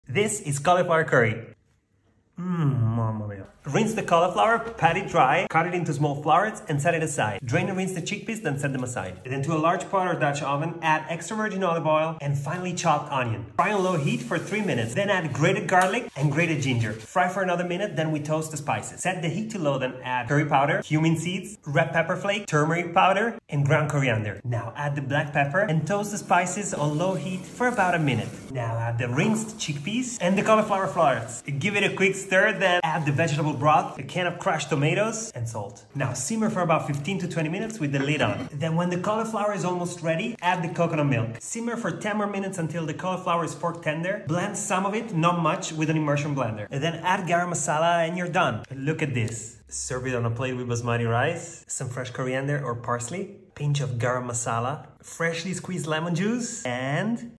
Cauliflower Curry is a veggie-packed Dinner Idea
⭐️  Cauliflower curry is a tasty, cozy, and easy one-pot recipe you can make with a few simple ingredients in about half an hour.

Have this Indian-inspired dish as a weeknight dinner, or make it ahead for meal prep. This is wholesome comfort food packed with bold flavors and plenty of nutrients.

⭐️ Ingredients
1 tablespoons extra virgin olive oil
1 medium onion chopped
2 cloves garlic grated
1 teaspoon ginger grated
2 teaspoons curry powder
1 teaspoon cumin seeds or ground
⅓ teaspoon red pepper flakes
1 teaspoon turmeric powder optional
½ teaspoon ground coriander optional
½ teaspoon black pepper
1 teaspoon salt
1 large cauliflower (about 2½ pounds or 1.2 kg)
2 cans (15 oz each) chickpeas
2 cups vegetable stock
1 can (15 ounce) crushed tomatoes
½ to 1 can (14 ounce) coconut milk
1 teaspoon garam masala (optional)

Cooked by Nico
Filmed & Edited by Louise
Theplantbasedschool.com